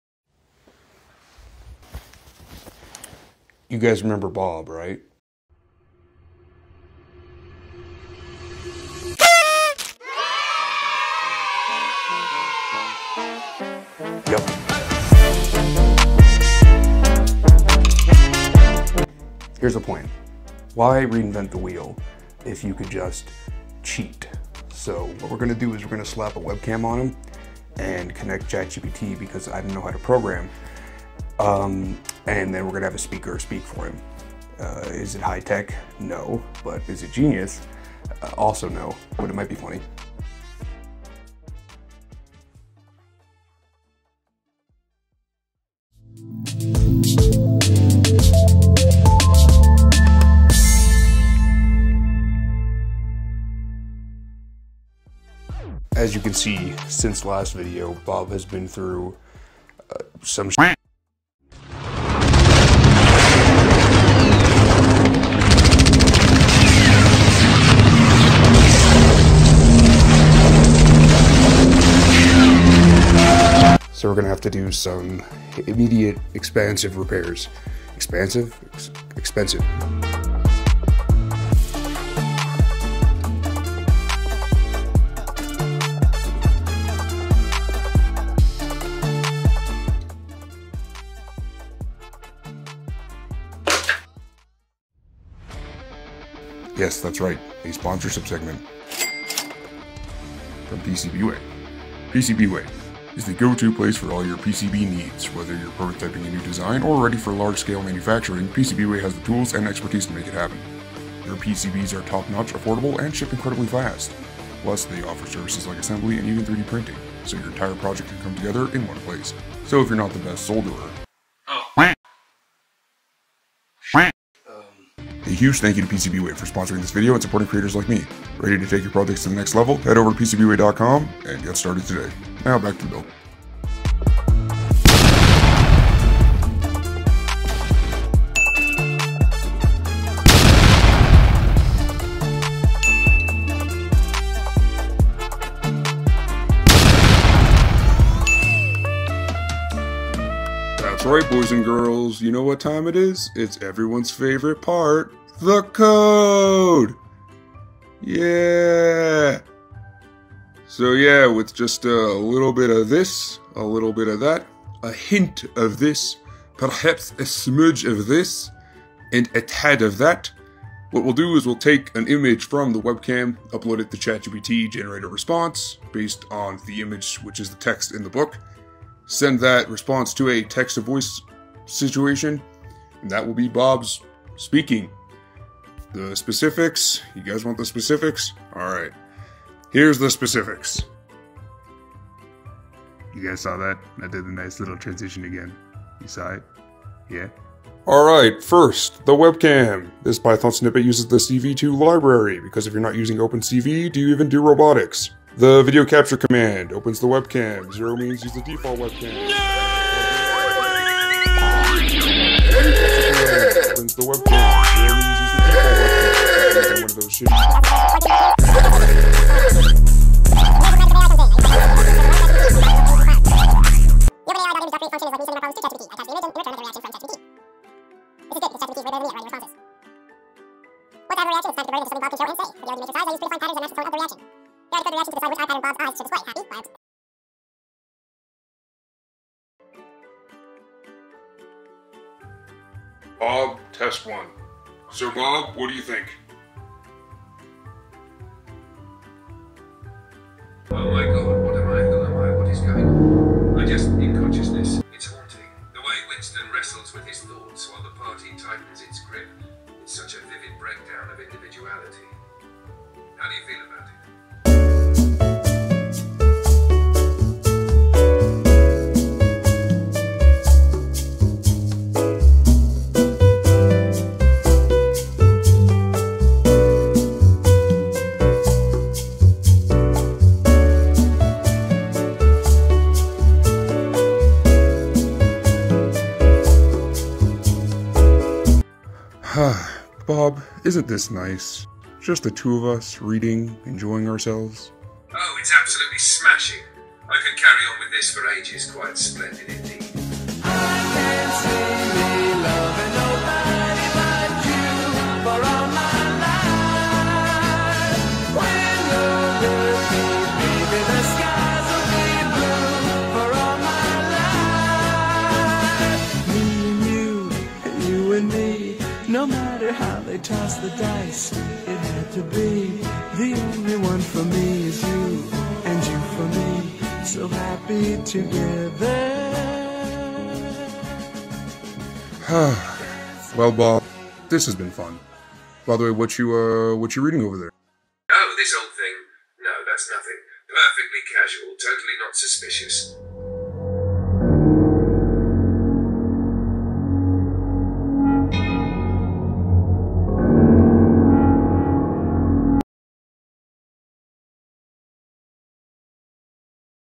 I made a robot to be my reading buddy using Chatgpt!
In todays video we'll upgrade bob to be able to read, react and speak using Chatgpt, using an Arduino, some motors and AI :) so if you like robotics, DIY or just in general like engineering content a like or a sub would be awesome!

Vecteezy for stock videos